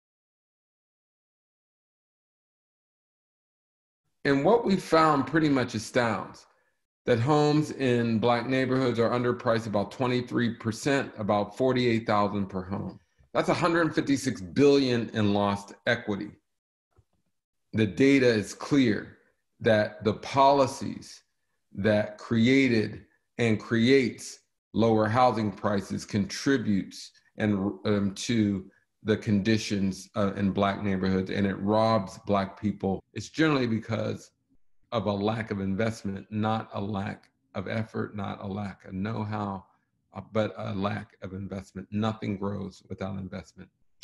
2020 Virtual ICBME: Week 3 Keynote Address Commercial
Tune in next Tuesday, October 20th, at 11:00 Central to hear Dr. Andre M. Perry speak on the devaluation of Black property and it's impact on the Black community.